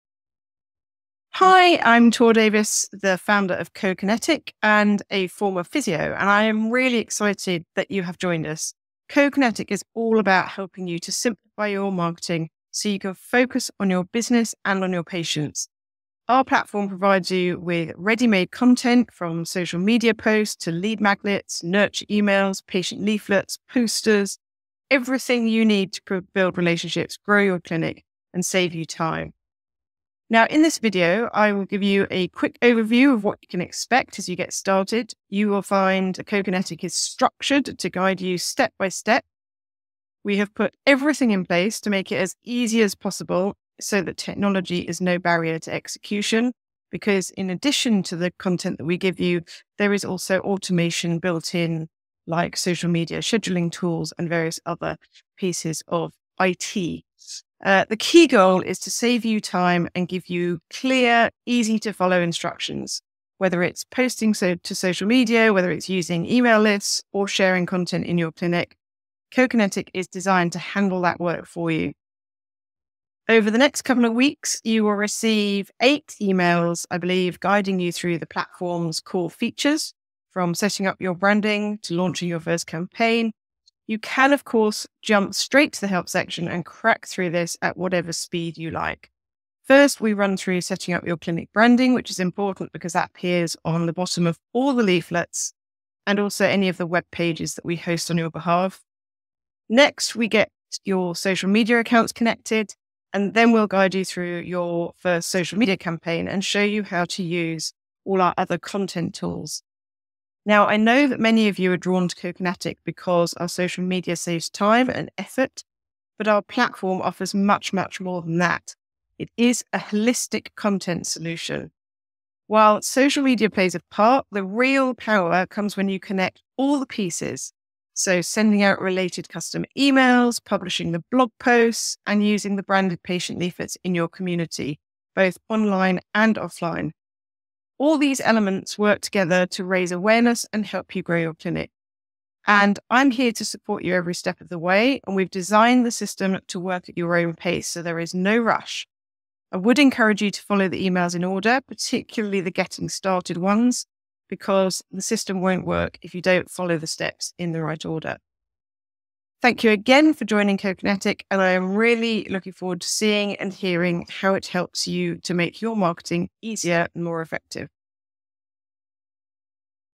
New Subscriber Welcome to Co-Kinetic: Simplifying Marketing for Physical Therapists
Join Tor Davies, founder of Co-Kinetic and former physiotherapist, as she introduces you to the Co-Kinetic platform designed to streamline your marketing efforts. This video provides an overview of the platform's features, including ready-made content like social media posts, emails, and patient leaflets. Learn how to set up your clinic branding, connect your social media accounts, and launch your first campaign. Co-Kinetic offers a holistic content solution, integrating social media, email campaigns, blog posts, and more to help you grow your clinic efficiently. Follow the step-by-step instructions and leverage the built-in automation tools to save time and focus on your business and patients.

00:00 Welcome to Co-Kinetic
00:30 Overview of Co-Kinetic Features
00:35 Step-by-Step Guidance
00:39 Automation and Tools
01:11 Email Guidance and Help Section
01:27 Setting Up Your Clinic Branding
01:37 Connecting Social Media Accounts
01:40 Launching Your First Campaign
01:55 Holistic Content Solution
02:17 Support and Encouragement
02:33 Final Thoughts and Thanks